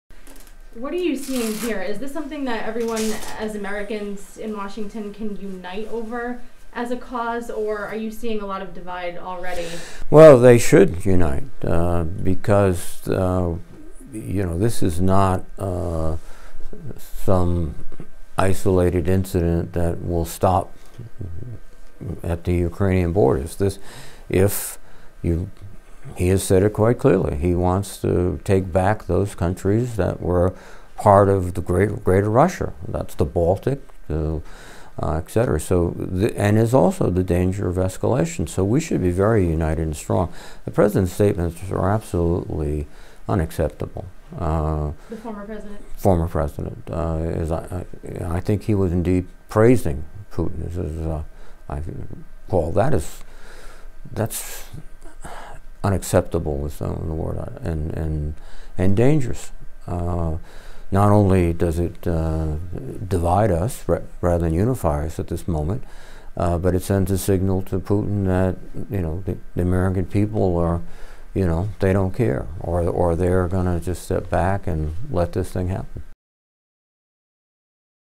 VIDEO NOW: Sen. Reed on Washington's reaction to Russian invasion
Sen Jack Reed discussed Russia's invasion of Ukraine on Thursday, Feb. 24.